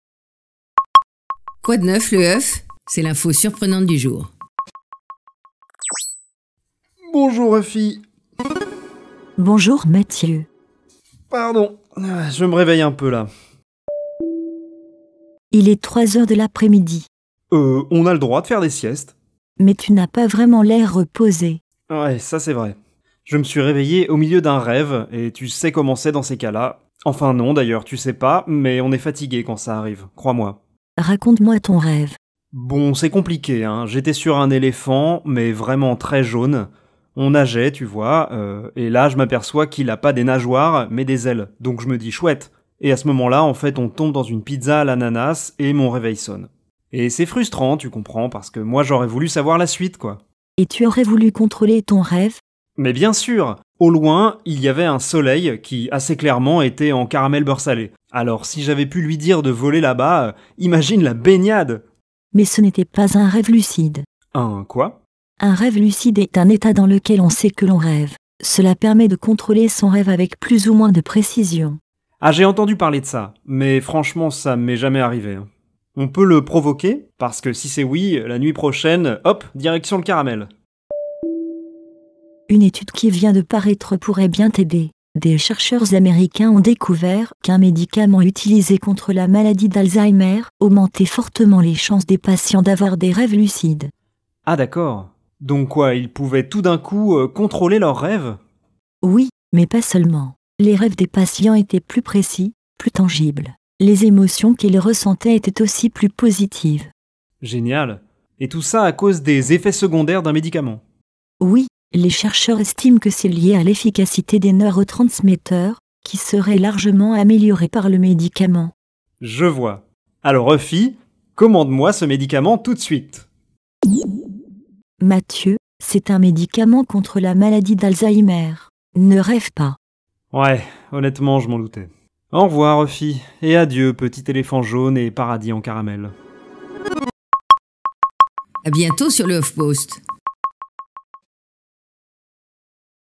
Un médicament pour contrôler ses rêves ?
De nombreuses méthodes existent déjà pour contrôler ses rêves, mais bien peu ont donné des résultats aussi convaincants. Le galantamine, un principe actif présent dans des médicaments luttant contre l'avancée de la maladie d'Alzheimer, pourrait selon une étude américaine permettre de vivre des rêves lucides.